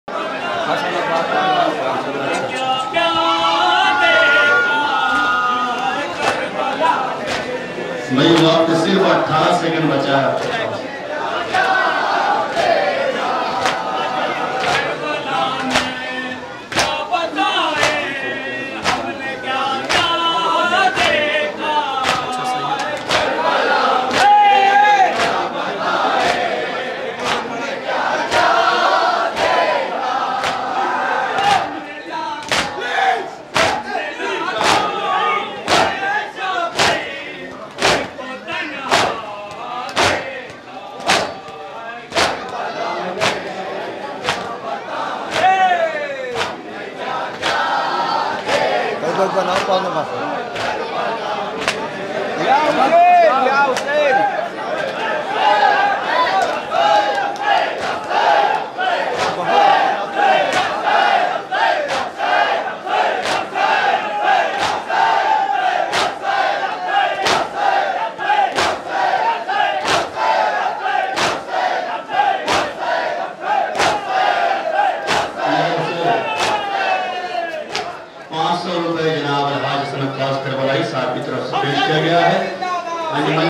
Sada e Matam | Anjuman Jafariya Jafrabad | 18 Safar 2022 Jafrabad Jalalpur
Date:
18 Safar 1444
16 September 2022

Venue:
Jafrabad (Chauhatta) Jalalpur Ambedkar Nagar Uttar Pradesh India

Zere Ehtimam:
Anjuman Azadar-e-Husaini (Shiya Youth) Ambedkar Nagar Uttar Pradesh India

Organized By:
Momimeen Jafrabad Jalalpur Ambedkar Nagar Uttar Pradesh India _____________________ ____________________